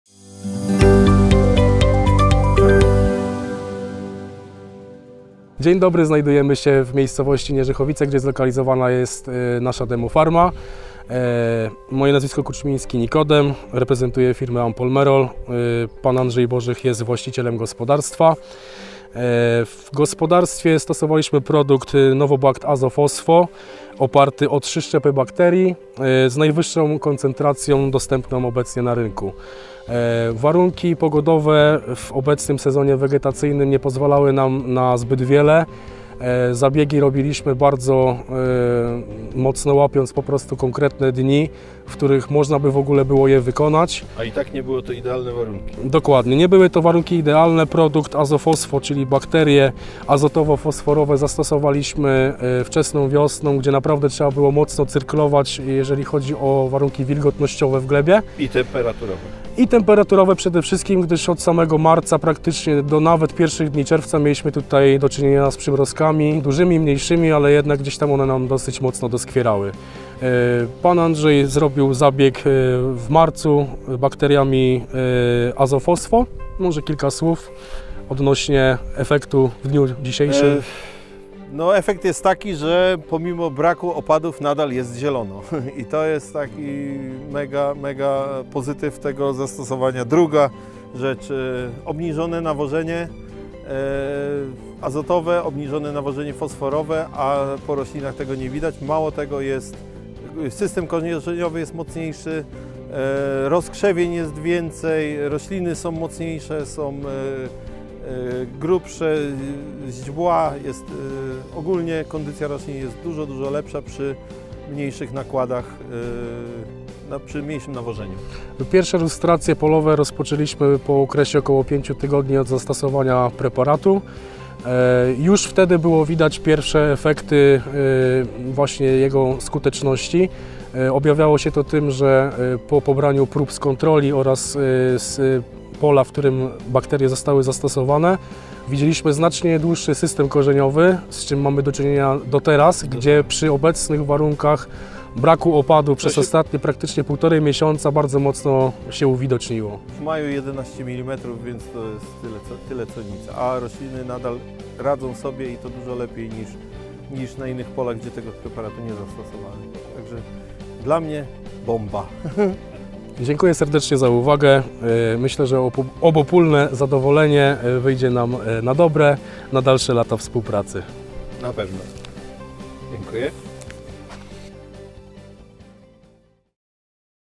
Efekty Novobakt AzoFosfo w Nieżychowicach
Na Demo Farmie w Nieżychowicach spotkaliśmy się z właścicielem gospodarstwa - Panem Andrzejem Borzychem. Wspólnie oceniliśmy i efekt zastosowania preparatu Novobakt AzoFosfo na Jego uprawach.